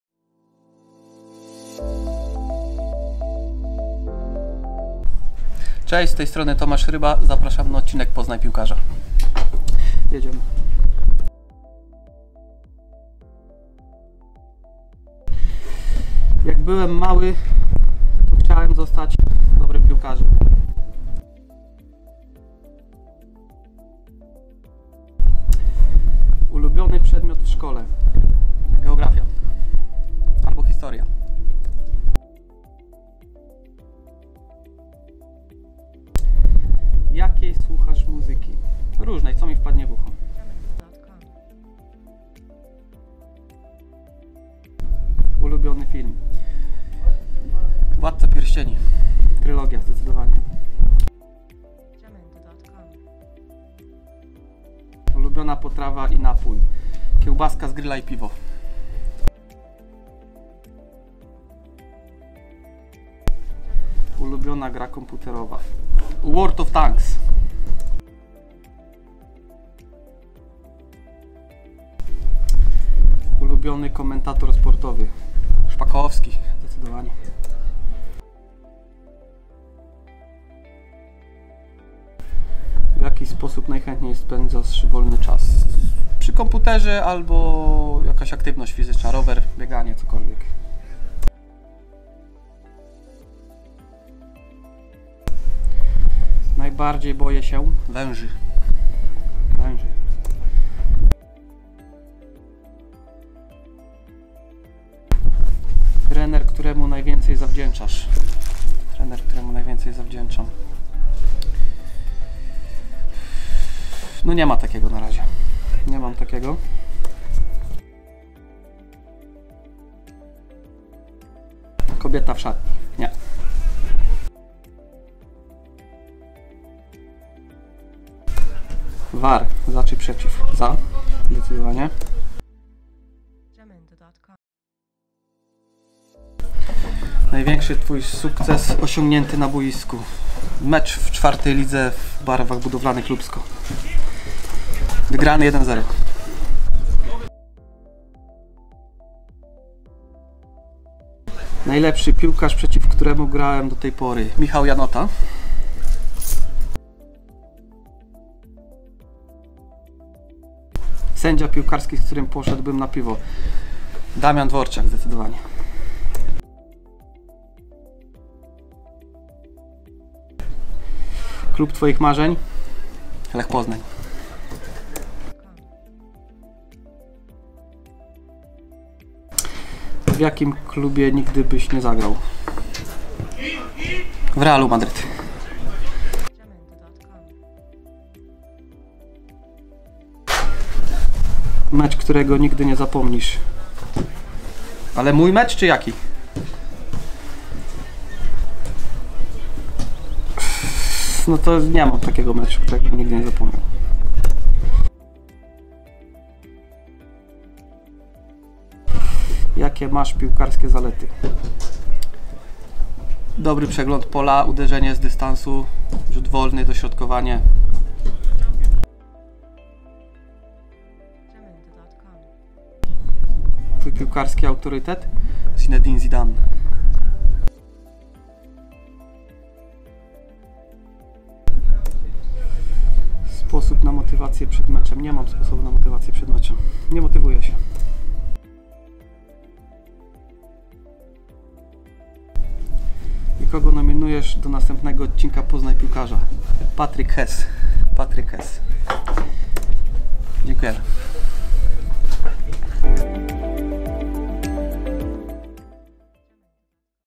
Poznaj piłkarza - Tomasz Ryba
Rozpoczynamy nowy cykl programów pod hasłem ,,Poznaj piłkarza”. Będziemy w nim przedstawiać piłkarzy Sparty Mierków trochę od innej strony. Pierwszym zawodnikiem, którego przepytaliśmy był Tomasz Ryba. Sprawdźcie jak poradził sobie z pytaniami!